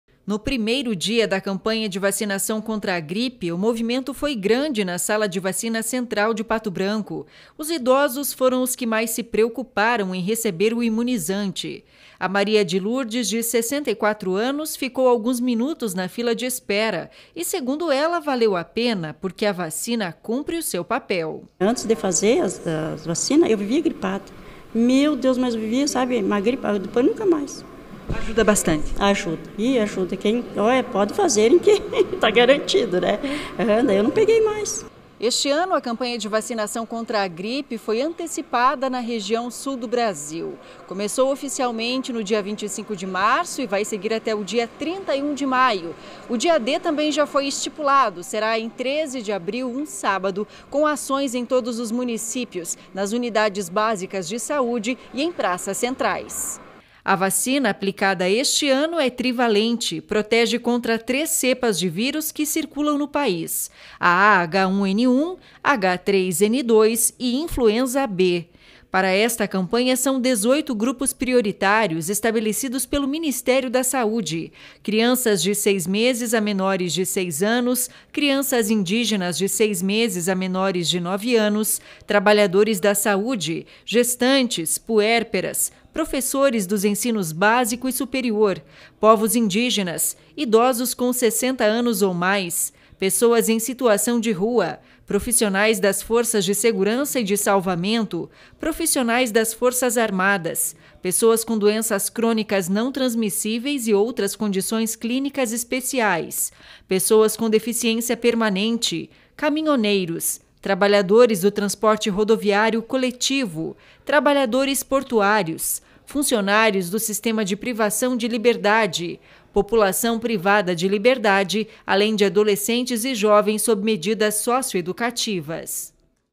Primeiro dia de vacinação contra a Influenza tem grande procura em Pato Branco
Saúde: A campanha nacional de vacinação contra a Influenza começou nesta segunda-feira e segue até o dia 31 de maio. Podem receber a vacina 18 grupos considerados prioritários pelo Ministério da saúde.